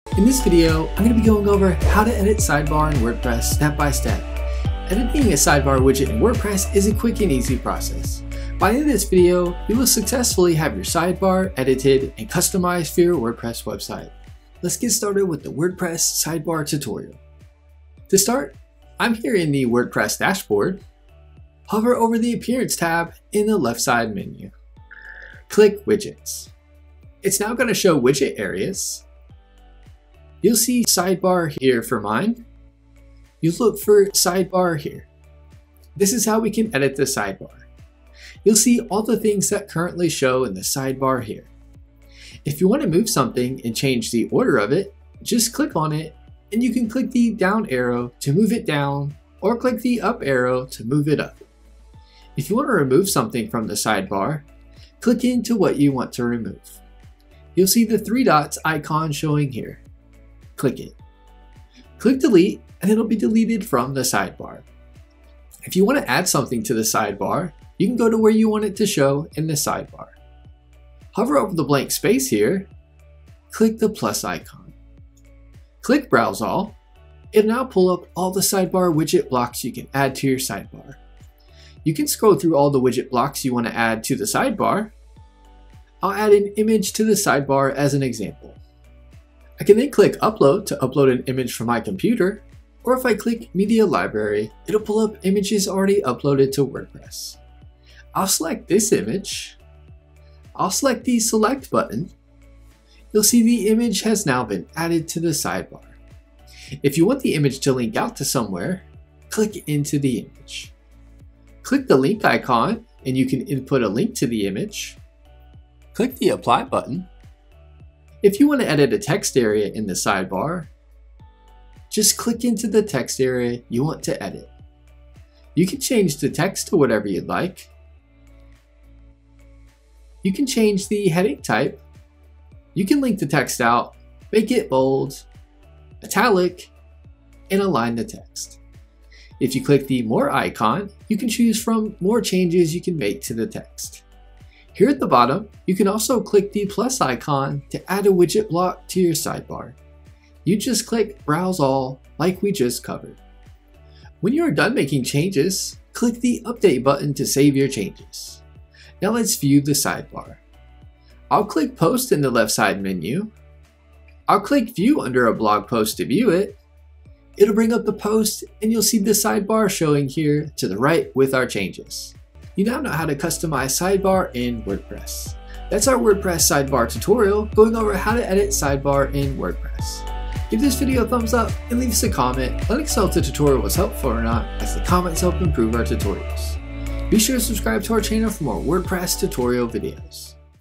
How To Edit Sidebar In WordPress 2026 🔥 - (FAST & Easy!)
Learn how to customize sidebar in WordPress step by step. Editing a sidebar widget in WordPress is a quick and easy process.

Let's get started with the WordPress sidebar tutorial. To start, I’m here in the WordPress dashboard. Hover over the Appearance tab in the left-side menu. Click Widgets. It’s now going to show widget areas. You’ll see sidebar here for mine. You’ll look for sidebar here. This is how we can edit the sidebar. You’ll see all the things that currently show in the sidebar here.

If you want to move something and change the order of it just click on it and you can click the down arrow to move it down or click the up arrow to move it up. If you want to remove something from the sidebar. Click in to what you want to remove. You’ll see the three dots icon showing here. Click it. Click delete and it’ll be deleted from the sidebar.

If you want to add something to the Sidebar. You can go to where you want it to show in the sidebar. Hover over the blank space here. Click the plus icon. Click Browse All. It’ll now pull up all the sidebar widget blocks you can add to your sidebar. You can scroll through all the Widget Blocks you want to add to the sidebar. I’ll add an image to the sidebar as an example. I can then click Upload to upload an image from my computer or if I click Media Library. It’ll pull up images already uploaded to WordPress.

I’ll select this image. I’ll click the Select button. You’ll see the image has now been added to the sidebar. If you want the image to link out to somewhere click into the image. Click the link icon and you can input a link to the image.

If you want to edit a text area in the sidebar just click into the text area you want to edit. You can change the text to whatever you’d like. You can change the heading type. You can link the text out, make it bold, italic, and align the text. If you click the more icon you can choose from more changes you can make to the text.

Here at the bottom you can also click the plus icon to add a widget block to your sidebar. You’d just click Browse All like we just covered.

When you are done making changes click the Update button to save your changes. Now let’s view the sidebar. I’ll click Posts in the left-side menu. I’ll click View under a blog post to view it. It’ll bring up the post and you’ll see the sidebar showing here to the right with our changes.

You now know how to customize sidebar in WordPress. That’s our WordPress sidebar tutorial going over how to edit sidebar in WordPress.

Help With WordPress:
Fiverr: